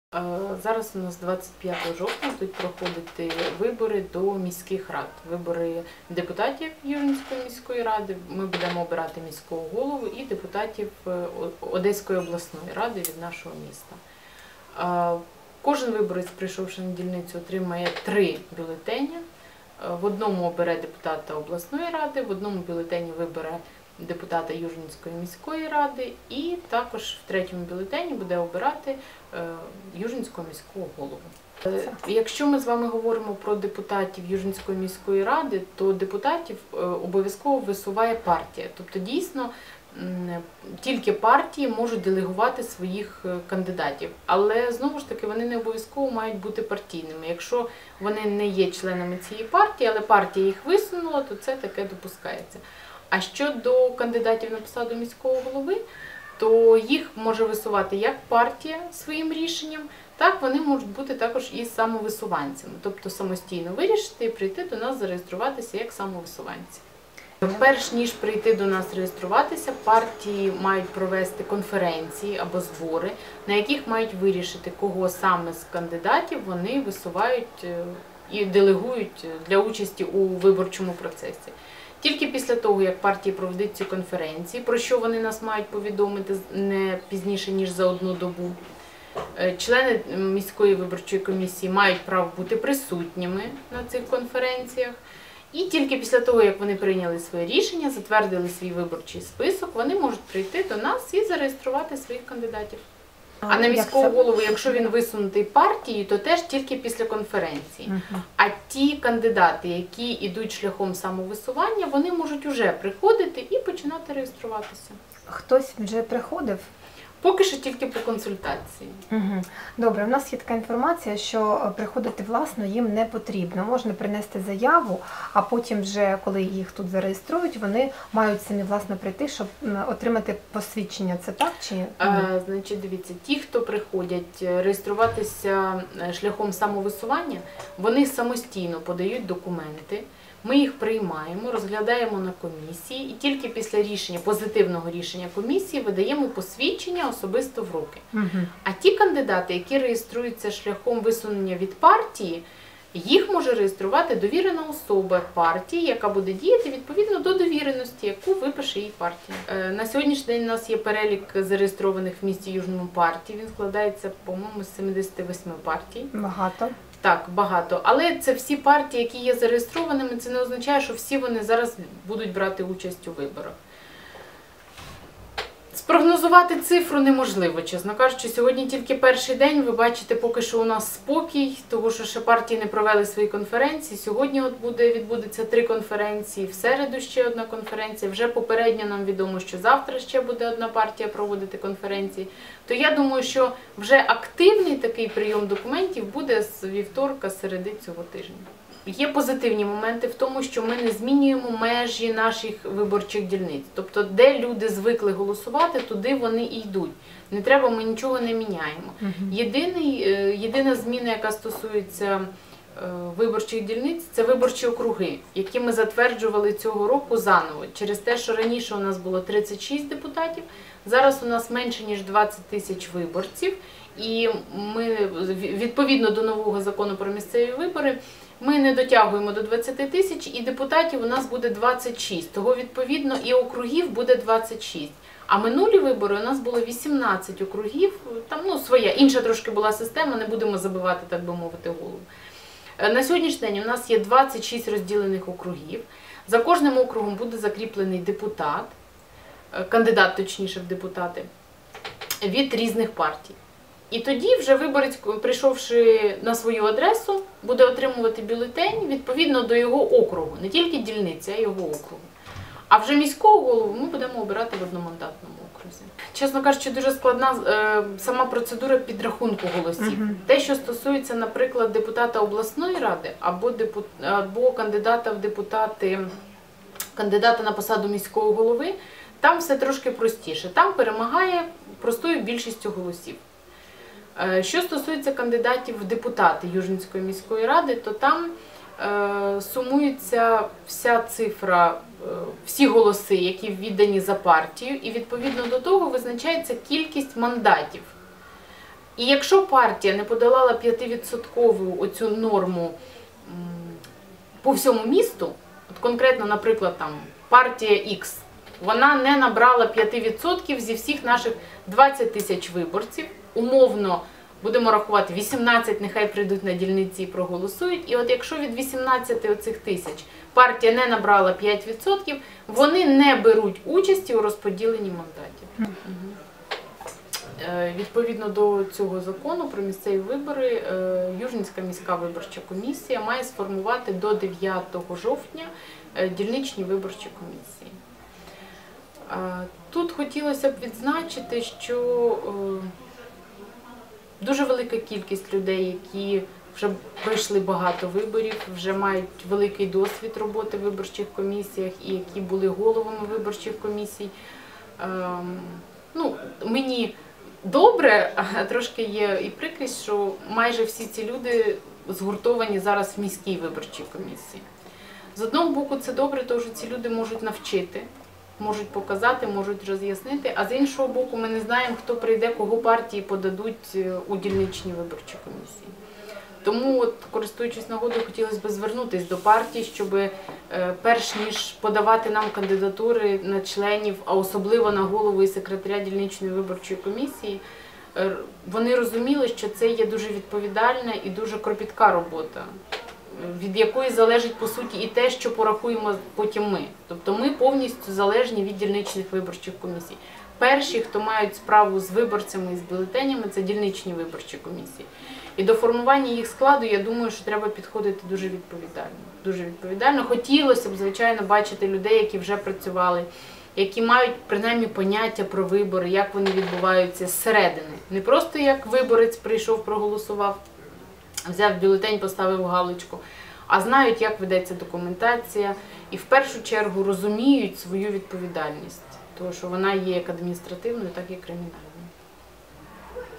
про процедуру виборів у Южному - Олена Баранецька
Інтервью із головою виборчої комісії міста Южного. Про те, як мають проходити вибори у місцеву та обласну ради.